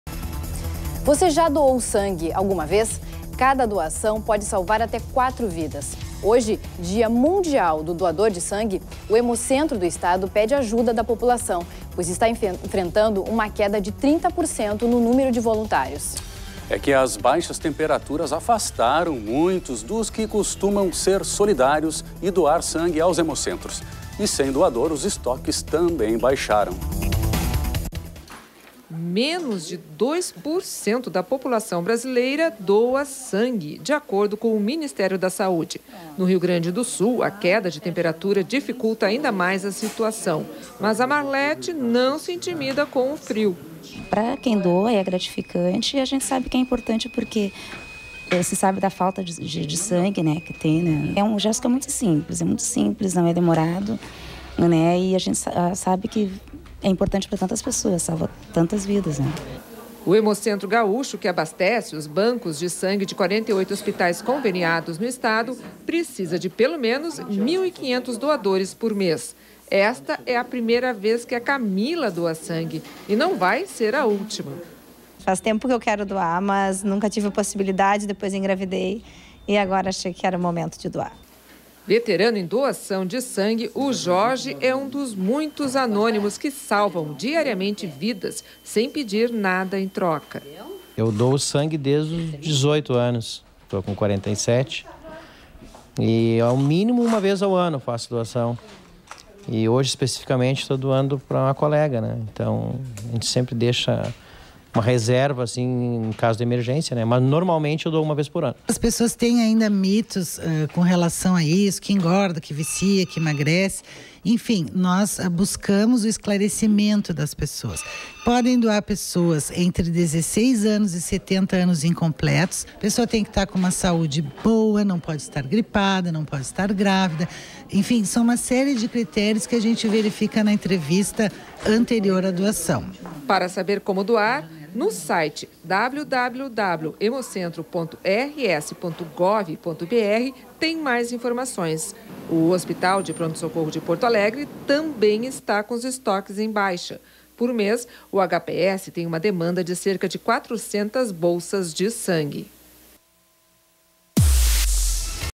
Diminui Doação de Sangue no RS -14/6/2016 | Jornalismo TVE RS
O banco de sangue do RS enfrenta uma diminuição de 30% no número de voluntários. A causa provável é o forte frio que se abate sobre o Rio Grande do Sul, que inibe os doadores.